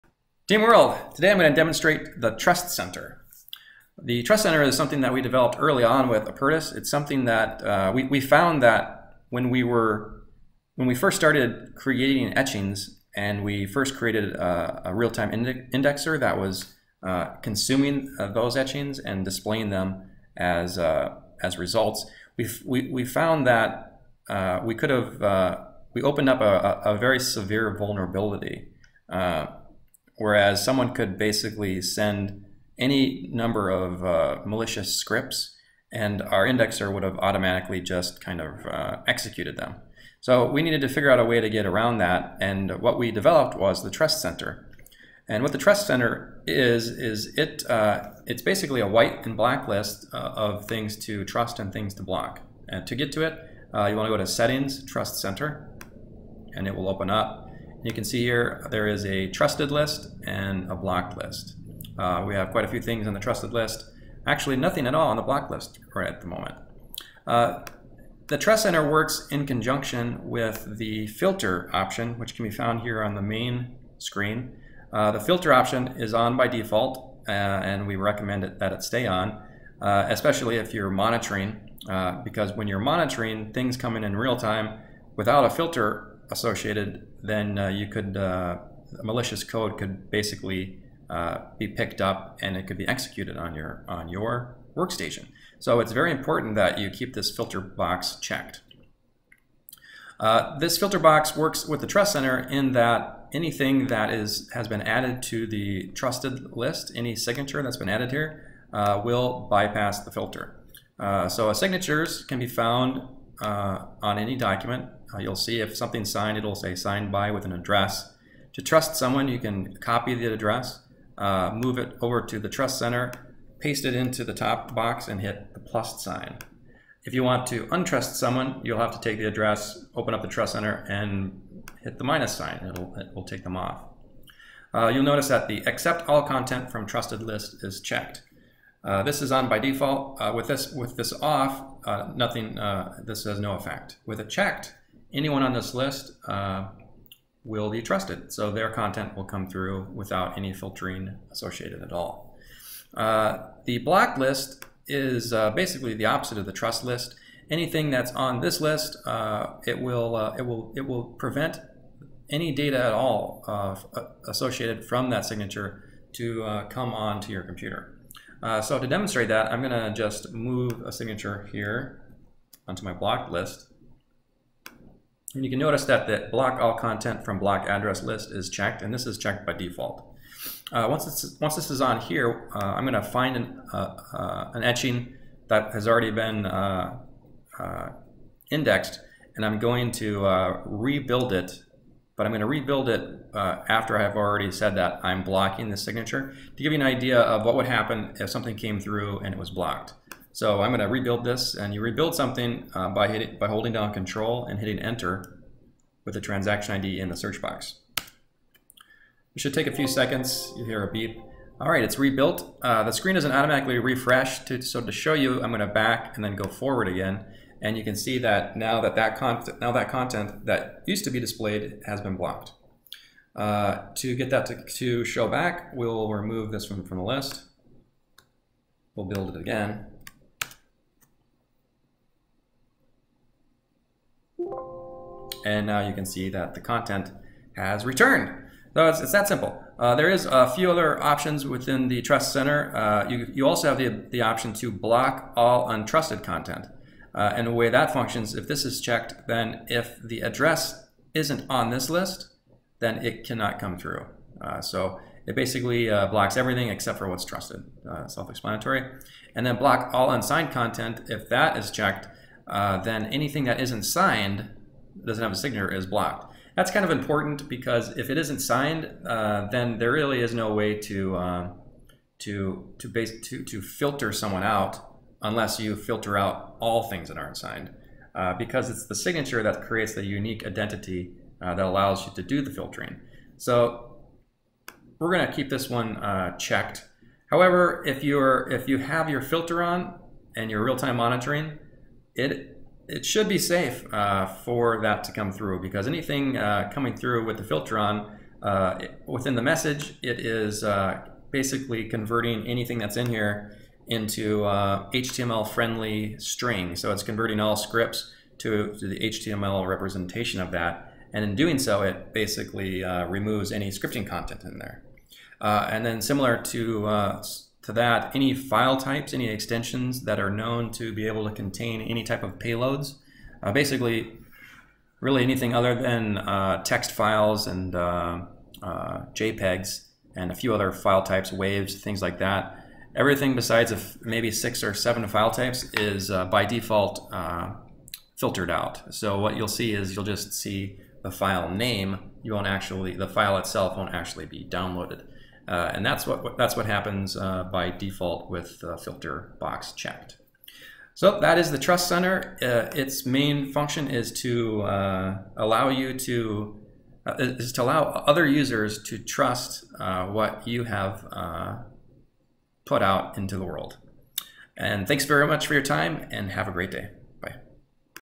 Apertus 0.3 Demonstration of Trust Center
Demonstration of the Apertus trust center.  The trust center brings trust back to a trustless network by using your wallets built in signature functions